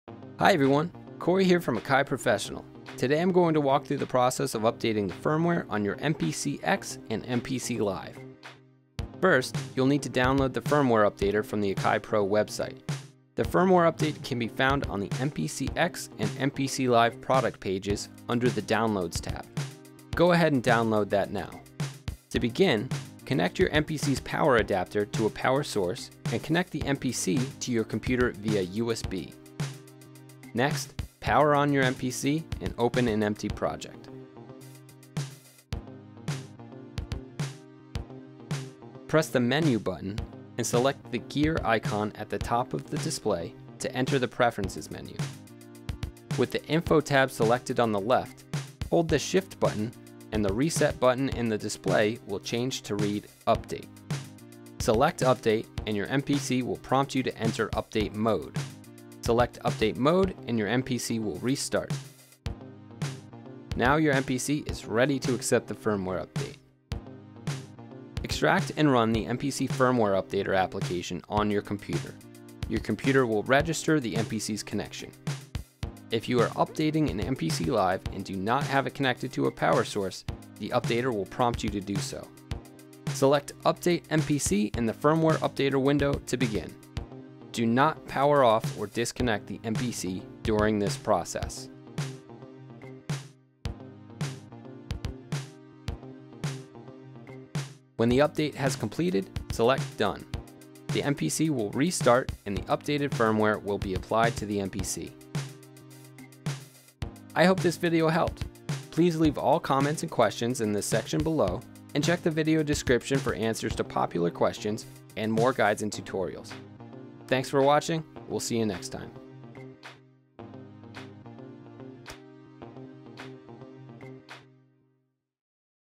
Akai Pro MPC X and MPC Live - Firmware Update Walkthrough
In this video, we walk through the process of updating the firmware on an MPC X and MPC Live to the latest update. This includes where to download, how to connect the MPC, and how to run the firmware updater.

Update:

The latest v2.0.5 firmware update for the MPC X and MPC Live was released on Sept. 27, 2017. Downloads can be accessed through the MPC Live and MPC X product pages, beneath the Downloads tab: